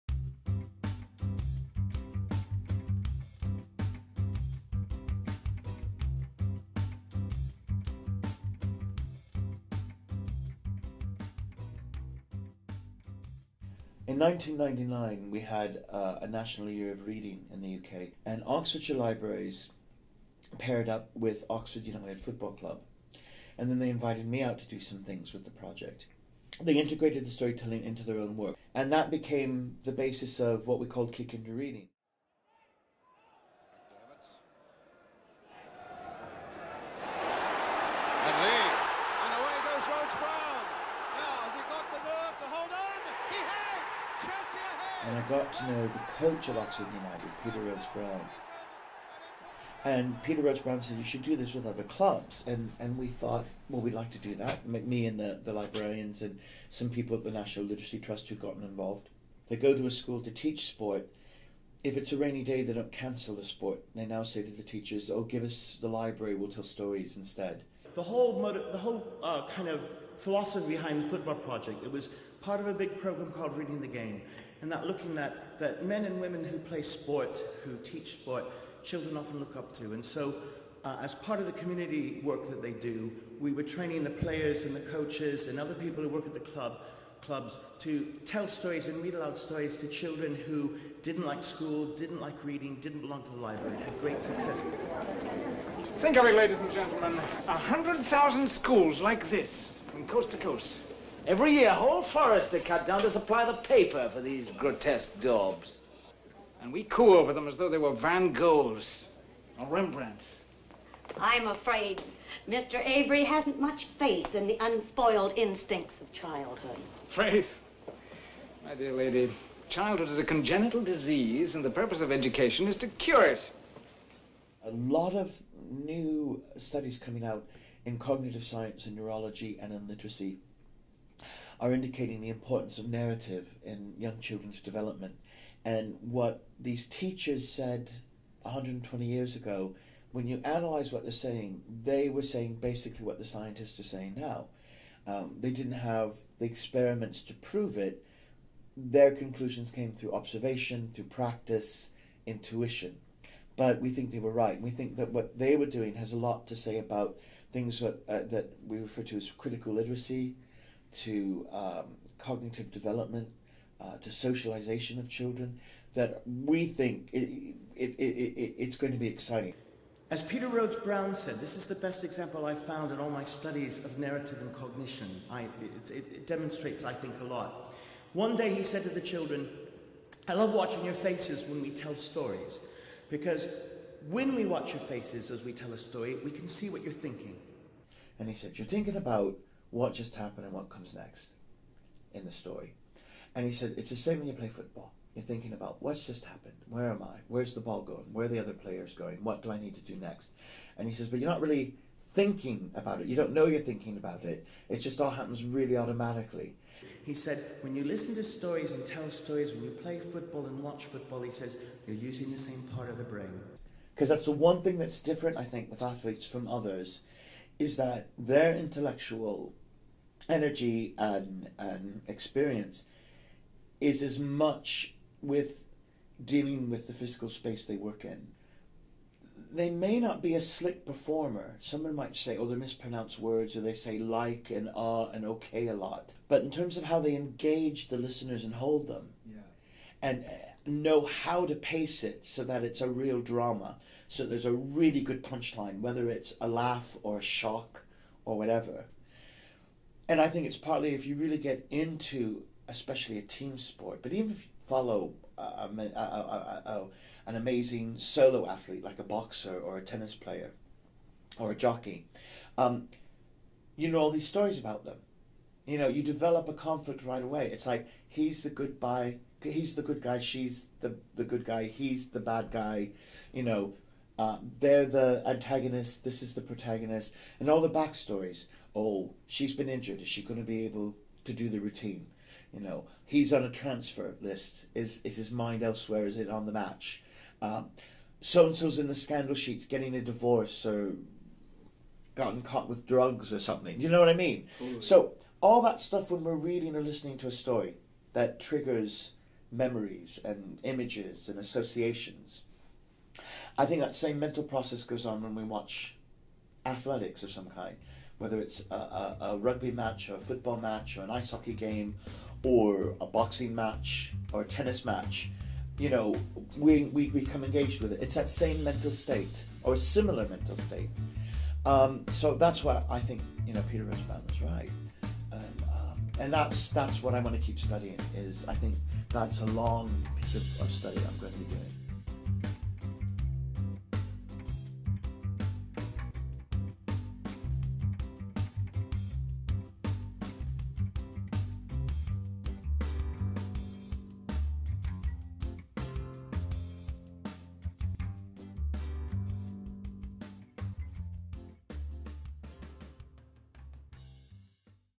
CCWOC::: Pat Ryan's interview about Sports and Stories
Do athletes and storytellers approach the world in the same way?

Dr. Patrick Ryan, expert storyteller, explores this question.  Watch this short interview with the Centre for Creative Writing and Oral Culture's Fall 2011 Storyteller and Writer in Residence and learn about how the world of narrative and the world of sports can intersect and educate.

To learn more about the University of Manitoba's Centre for Creative Writing and Oral Culture (CCWOC) check out this